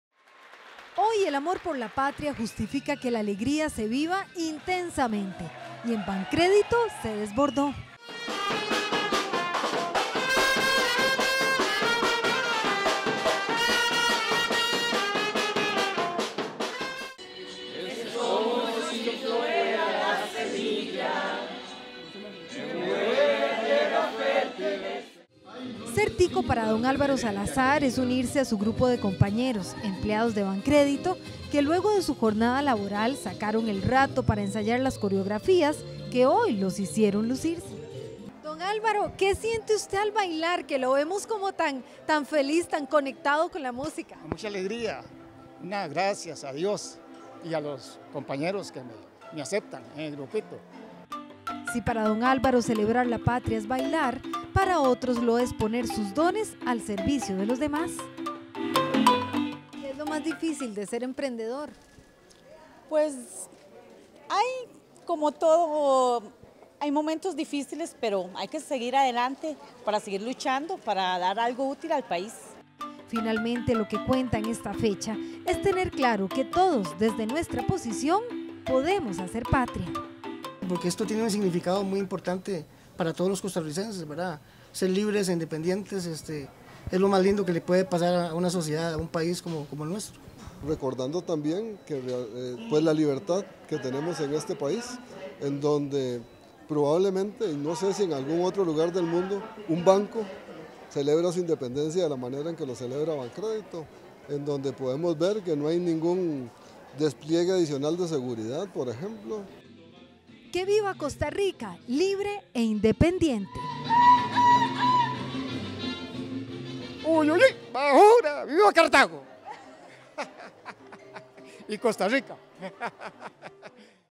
CELEBRACIÒN A LA TICA
Acostumbrados a convivir con números y con las palabras crédito, ahorro e intereses, este 13 de setiembre los empleados de Bancrédito rompieron la rutina y se vistieron de fiesta para celebrar los 192 años de independencia.

Hicieron participes de su fiesta a todos los clientes que ese día comieron también gallo pinto, arracache, pozol, chifrijo y cajetas de leche.